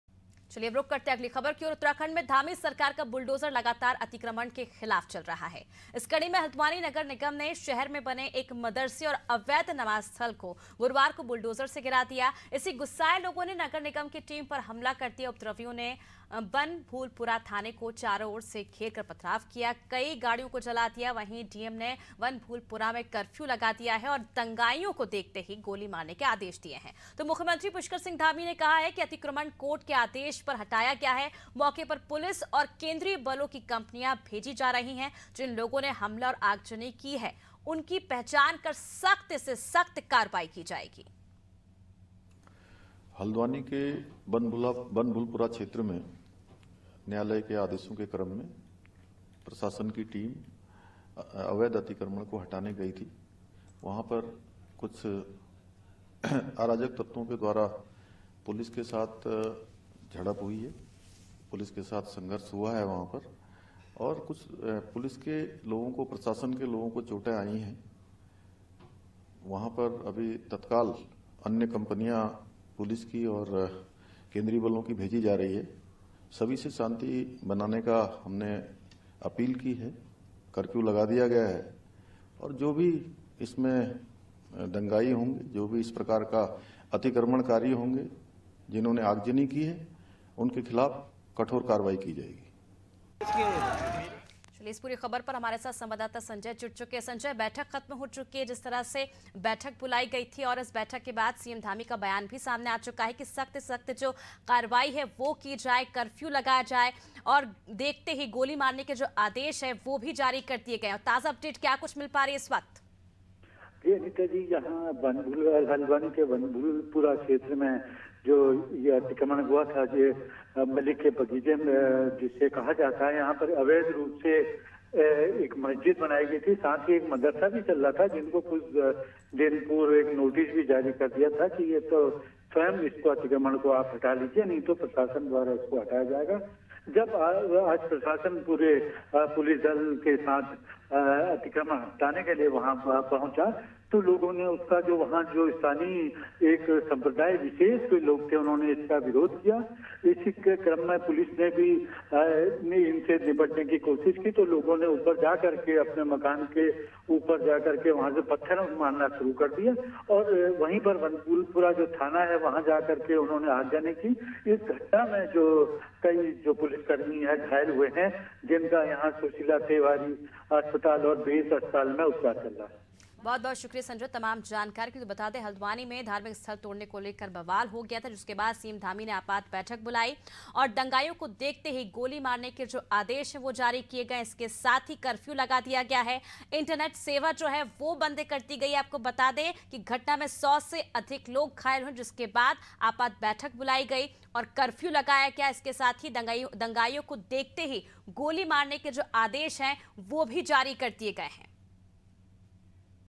Haldwani : अवैध मदरसे पर चला बुलडोजर, गुस्साए लोगों ने किया पथराव-आगजनी || Anaadi Tv
AIRTEL - 379